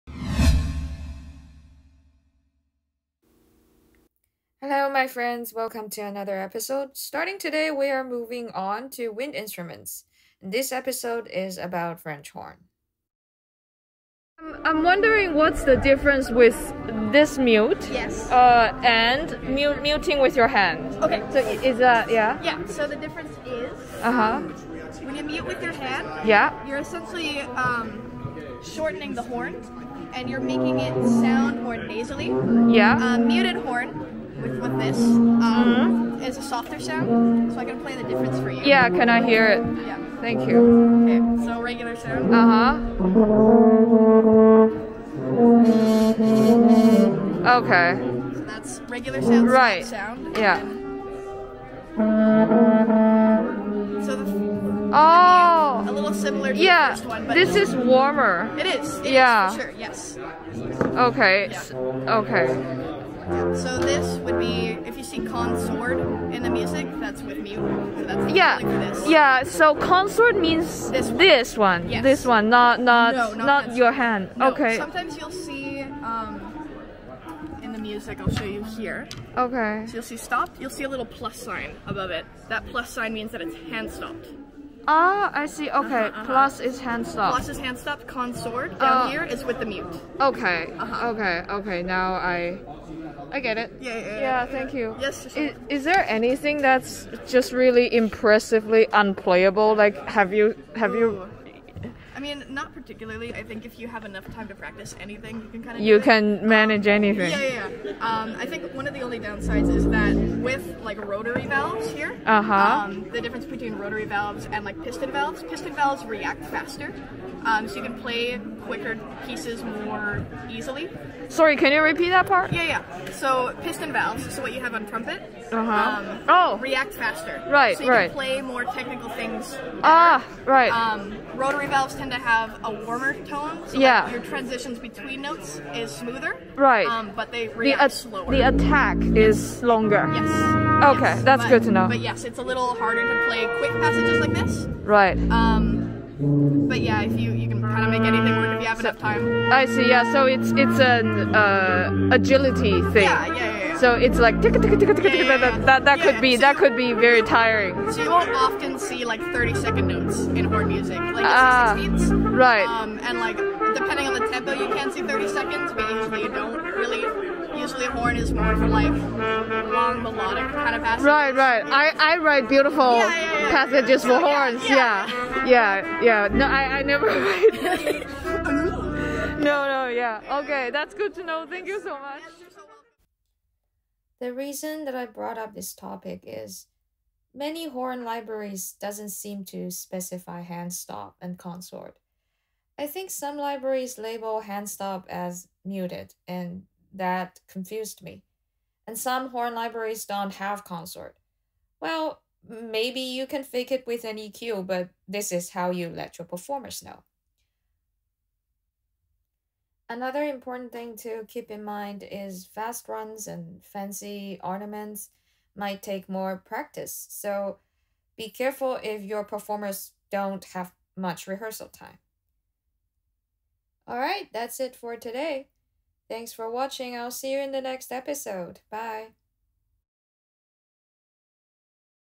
Behind the Scene #5 - French Horn
Hello composer friends,

Welcome to my Behind the Scene series! It's a series of short videos of me chatting with the live orchestral instruments and players, from a screen composer's perspective.

My goal is to develop specific understanding of each instrument and players instead of just textbooks.

I hope that could help us write more idiomatically and authentically, making the most out of them.

Here's the 6th episode - about french horn!